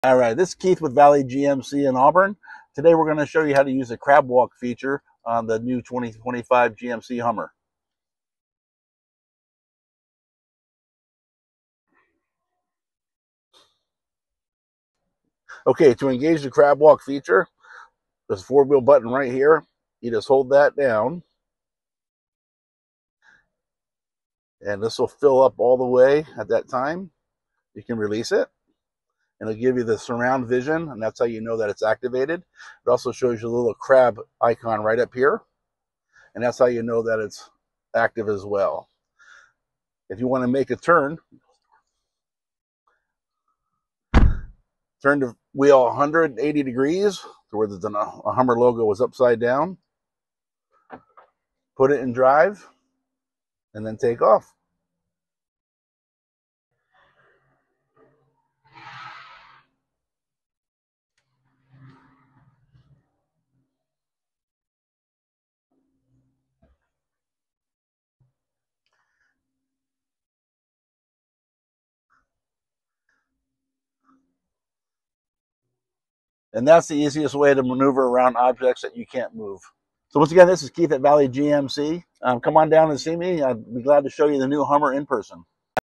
Crab Walk Demo on the GMC Hummer EV | Keith from Valley GMC Shows You How It Works!
Ever wondered how the GMC Hummer EV’s CrabWalk feature actually works? 🦀 Join Keith, one of our knowledgeable sales consultants here at Valley GMC, as he gives a quick and easy demo of this game-changing off-road innovation. From tight trails to tricky parking spots, CrabWalk gives you unmatched maneuverability and control — and you’ve got to see it in action to believe it!

🔧 Features Highlighted:

Hummer EV CrabWalk demonstration

Real-time steering angles of all four wheels

Tips on where and when to use it

Whether you're an EV enthusiast or just curious about next-gen truck tech, this feature is a must-watch!

📍 Visit us at 3104 Auburn Way North, Auburn WA 98002